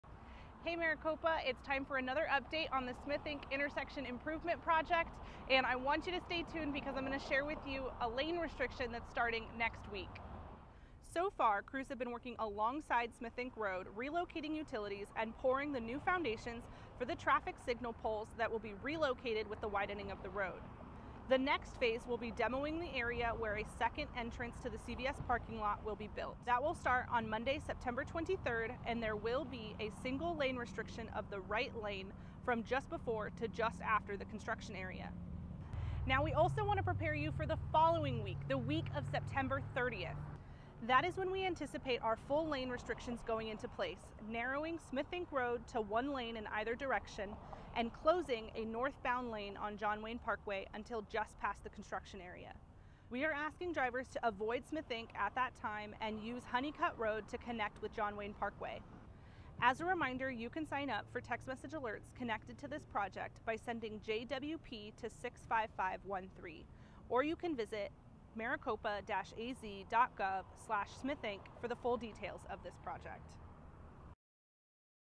Smith Enke and JWP Intersection Update - September 19 2024
Lane restrictions are planned to start at 5 a.m. on Monday, September 23. Crews will be demoing the area where a second entrance to the CVS shopping center will be built.

The lane restriction will remain in place until crews transition to the next phase of the project on Monday, September 30. At that time, lane restrictions will narrow Smith Enke Road to one lane in both directions and close a single northbound lane on John Wayne Parkway through the construction area.